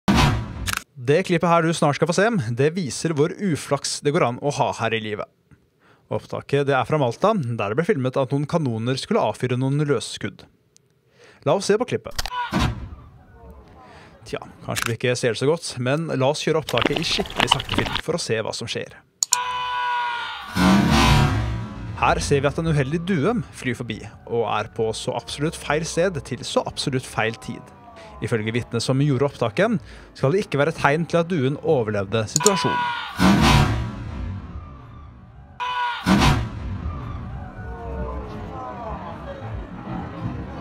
Først i sakte film ser man hva som går galt
UHELDIG: Med mange vitner tilstede gikk en saluttering på Malta ikke helt etter planen. Video: Newsflare